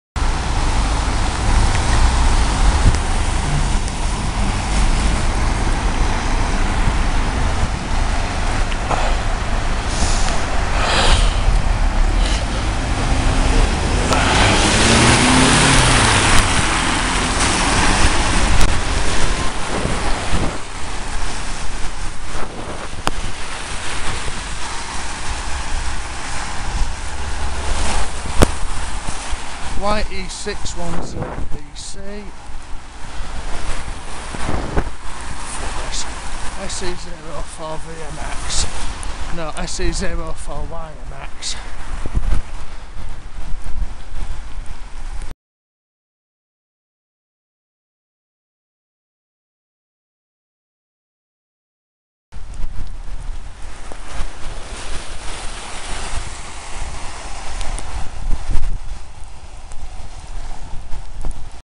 FL57VTC Close, impatient pass, YE61ZPC and SE04YMX - dangerous, close passes
Eventually I got sick of the crap overtakes, and rather than hover about just outside of the door zone I took primary.